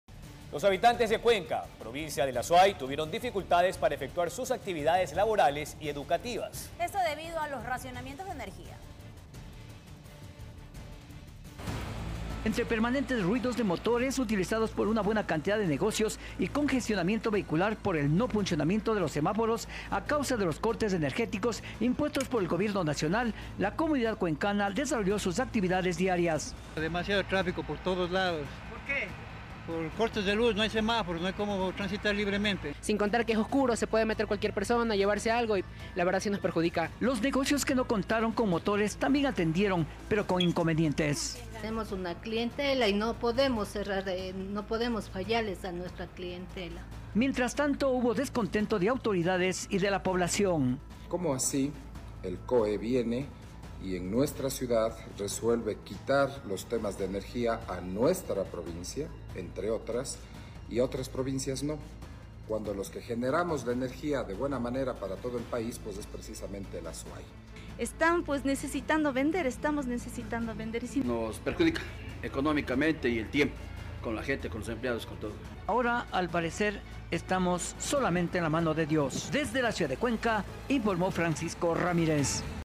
Cuenca presentó dificultades durante apagón
Los habitantes del lugar tuvieron varios inconvenientes para efectuar sus actividades laborales y educativas debido a los racionamientos de energía. Además de los locales mismos, el tráfico también se vio perjudicado por la ausencia de semáforos.